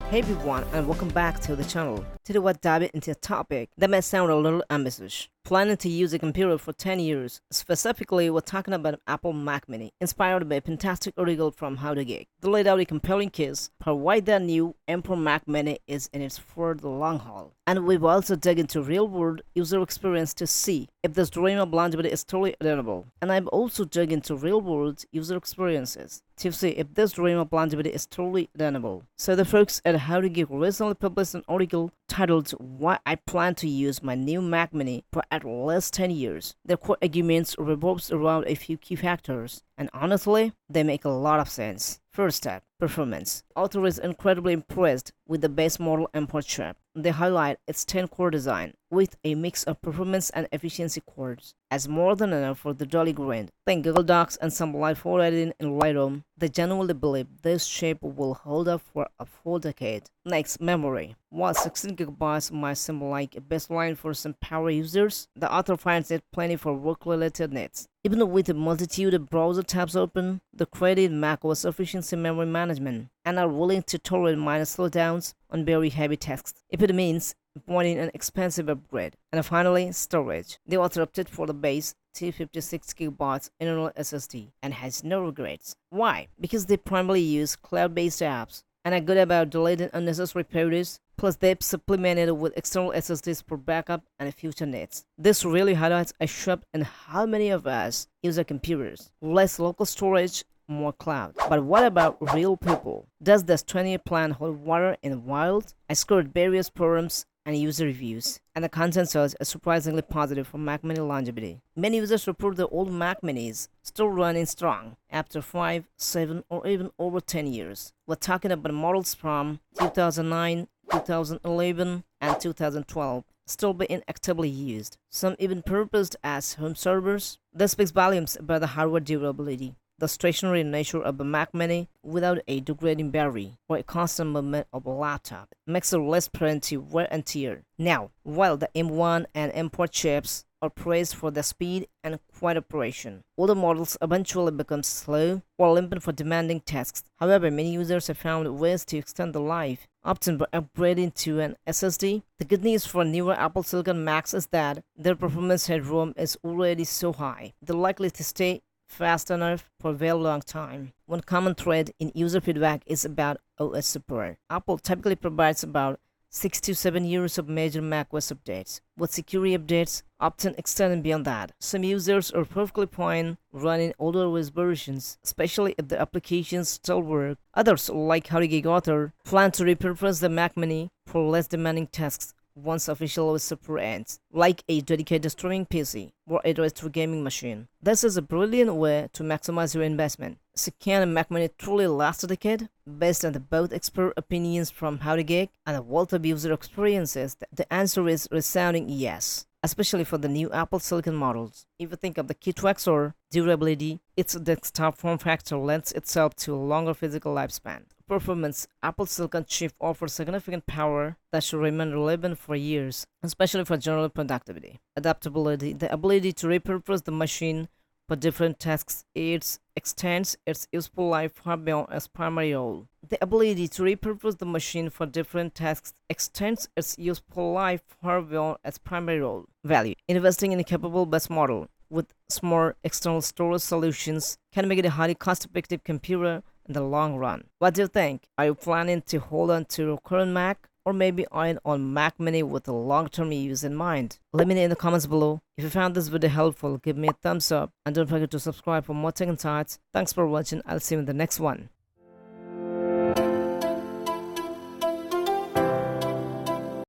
Mac Mini M4 Review: Can It Last 10 Years?
I’m testing the Mac Mini M4 as my daily driver for the next 10 years! 🚀
In this Mac Mini M4 review, you’ll see my setup, performance test, and first impressions of Apple’s latest Mac Mini with the M4 chip.

From 4K video editing in Final Cut Pro, multitasking, and productivity apps to everyday use, I’ll push this powerful desktop to its limits and find out if the Mac Mini M4 is future-proof for long-term use.

If you’re wondering about Mac Mini M4 performance, speed, value for money, or daily productivity, this video will help you decide if it’s the right computer for your setup.

👉 Question for you: Are you Team Mac or Team Windows – and why?